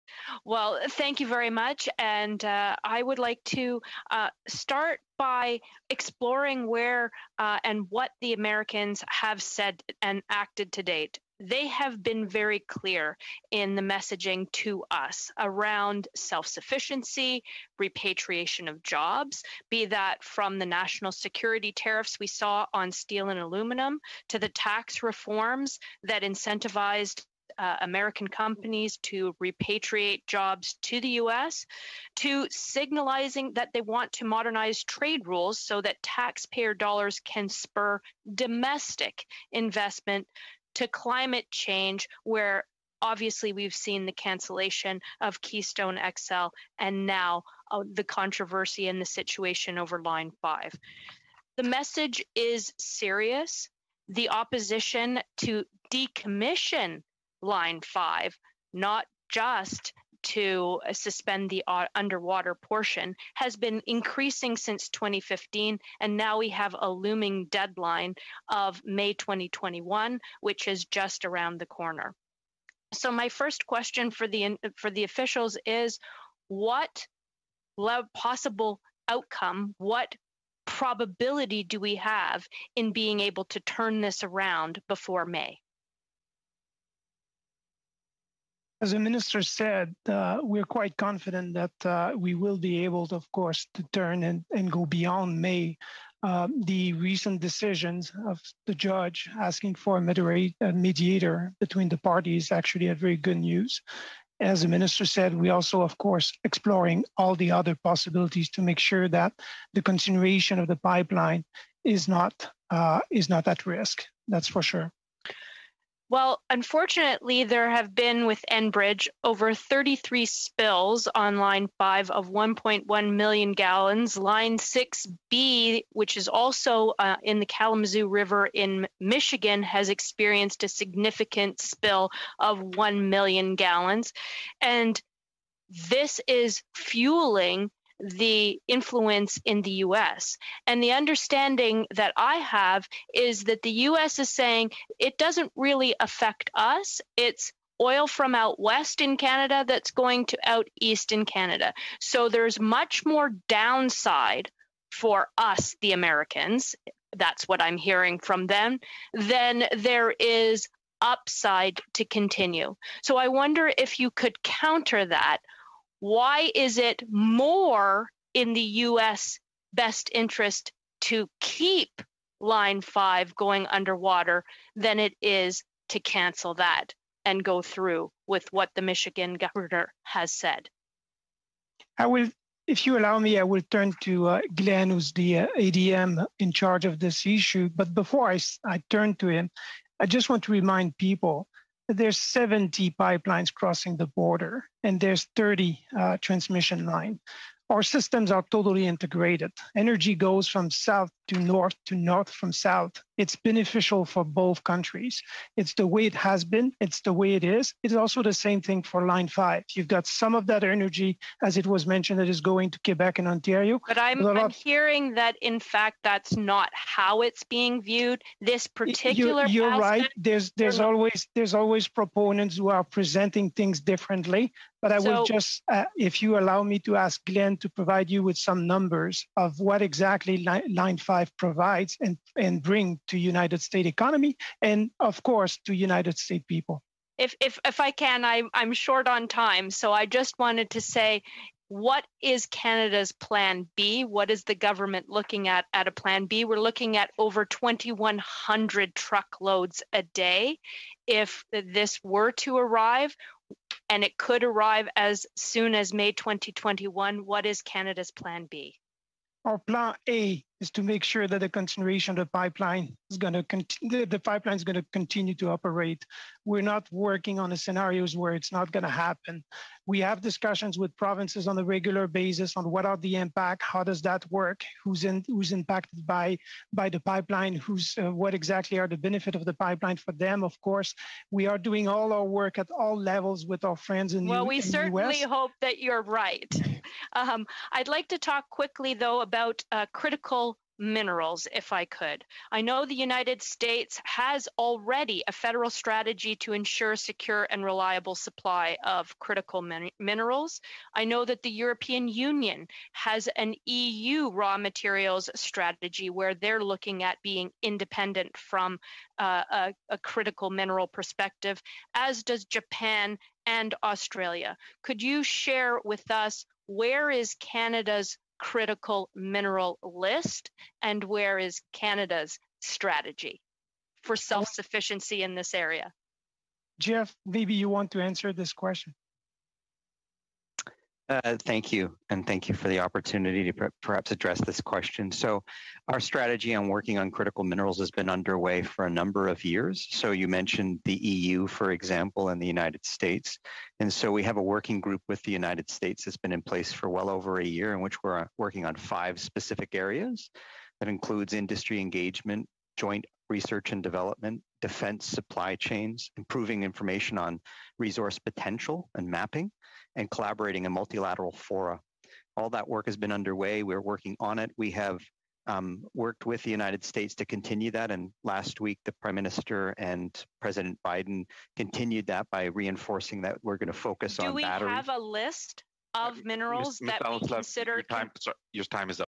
Canada- US Committee
The Americans have been clear in their messaging to us around their self-sufficiency and repatriation of jobs, from national security tariffs on steel and aluminum to tax reform. With the cancellation of Keystone XL and the controversy over Line 5, Canadians are wondering if Canada’s priorities have been shared with the US.